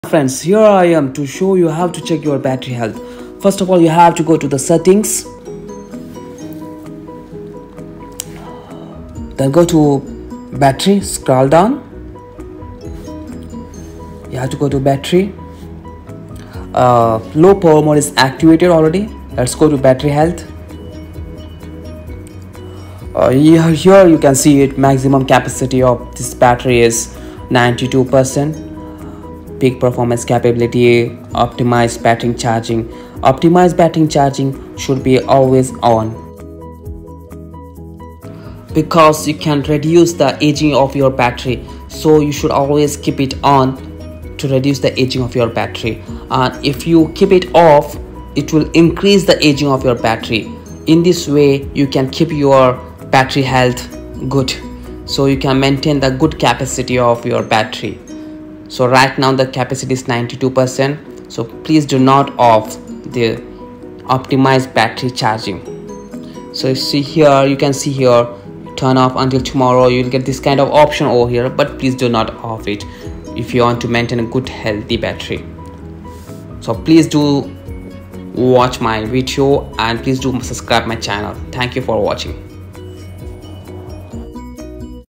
How to check battery health of your iPhone 11 Pro
You can also check the status of your iPhone 11 Pro battery.
In this video you can see the battery percentage of your battery and know how to switch on the optimised battery option to reduce the battery aging.

As the battery degrades, their maximum capacity reduces. An aging battery may cause performance slowdowns and you will also notice that your iPhone may not last as long on a single charge, as the total effective battery capacity is less.